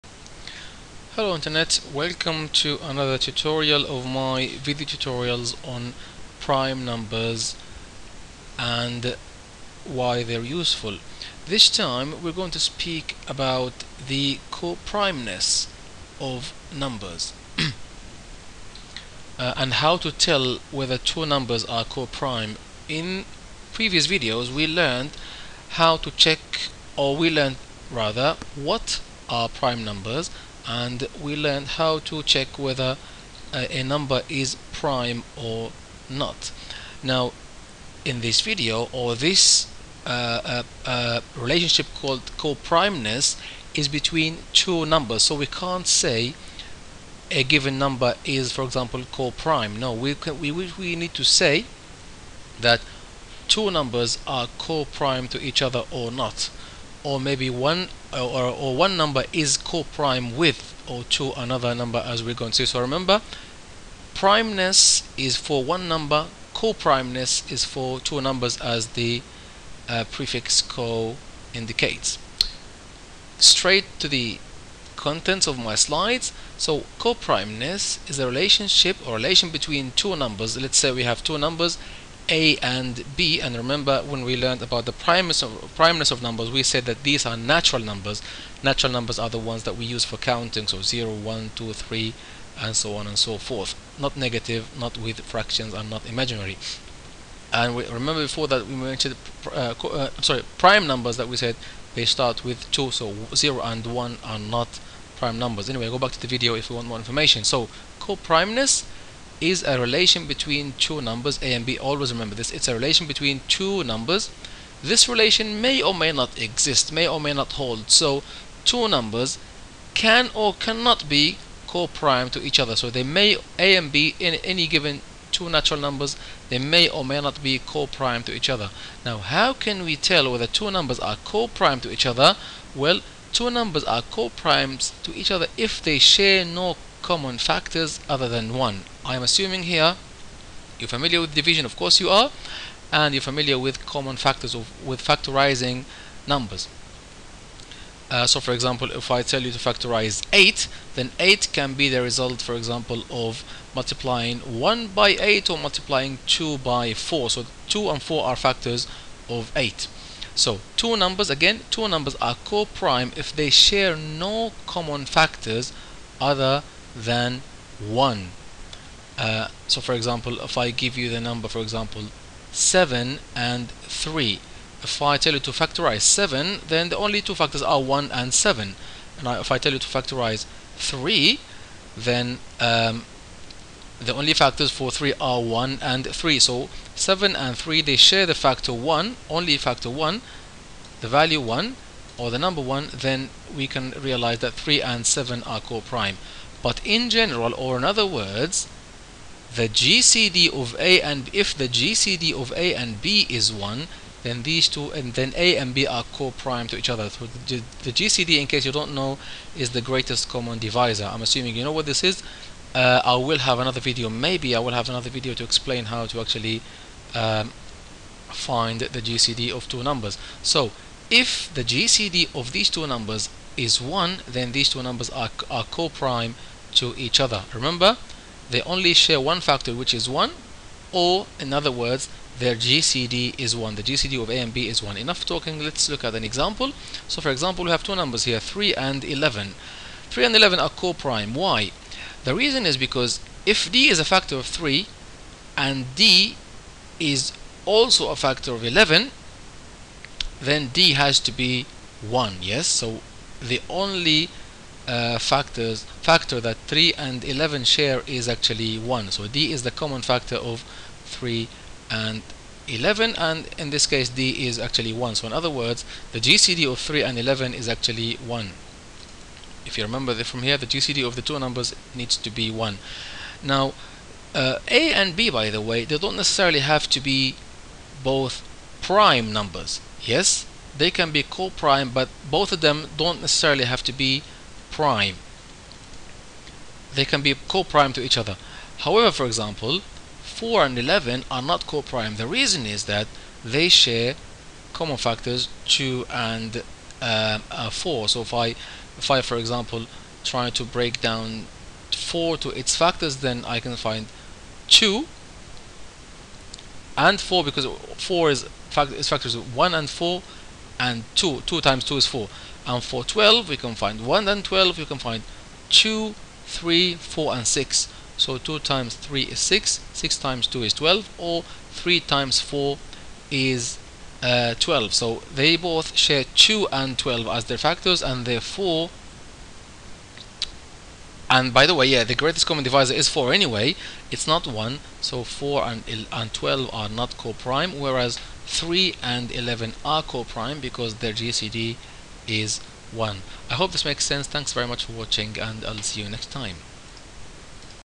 Coprimeness (Coprime Numbers): Check if two numbers are coprime
Here I explain what coprimeness is and how to tell if two numbers are coprime